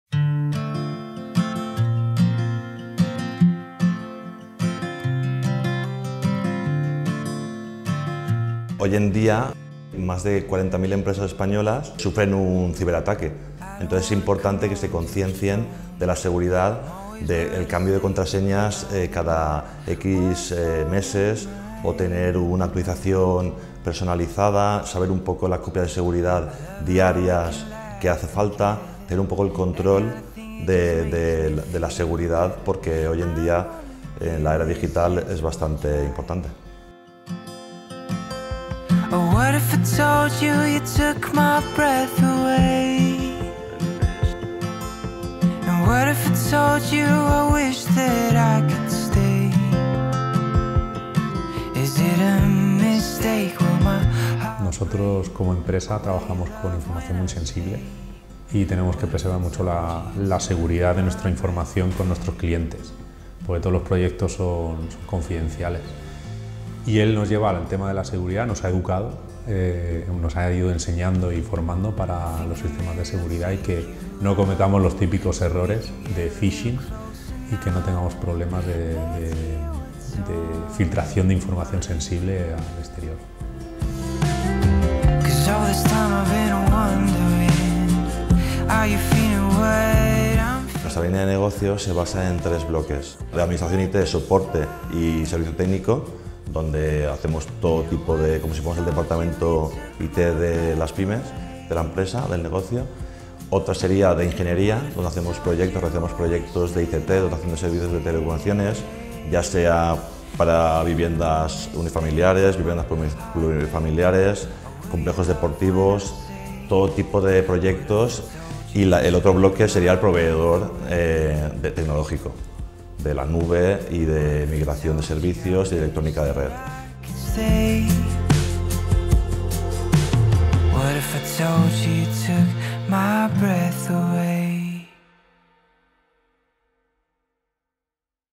PICEI2021 | IoTIC NETWORKS
IoTIC Networks, empresa ganadora en la encuesta PICEI 2021,  que coordina el Parc Científic de la Universitat de València (PCUV).